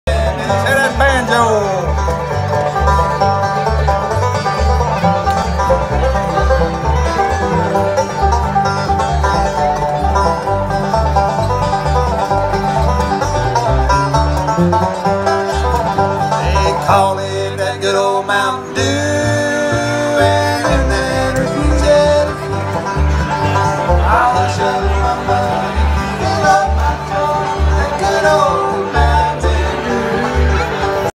Carolina Inn Fridays on the Front Porch
Phillip Clapp - Mandolin
Courtney Clapp - Banjo
Jeff Stickley - Guitar
Miles Andrews - Bass